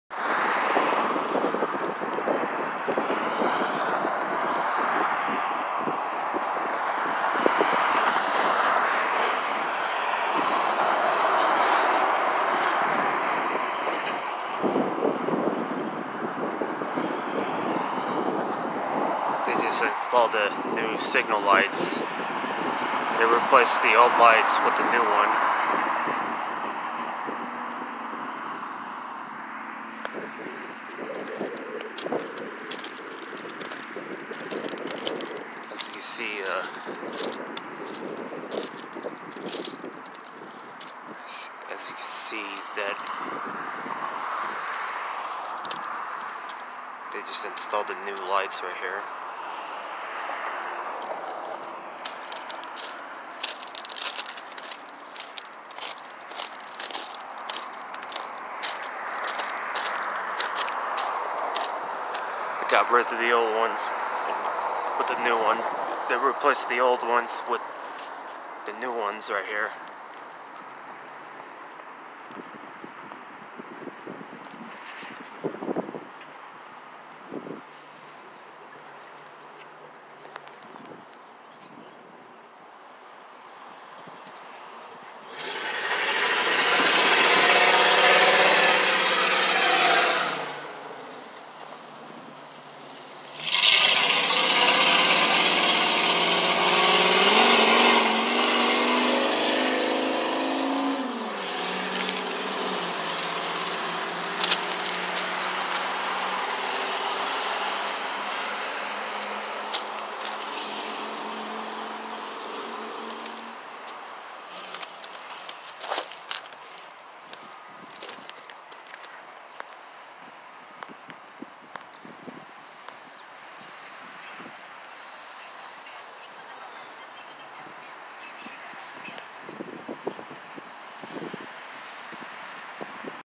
New Signal Light Lens @ Sacramento CA 4/19/2020
I went out to check out this new signal light lens and the old lens with the signal lights got replaced with the new one on the UP Martinez subdivision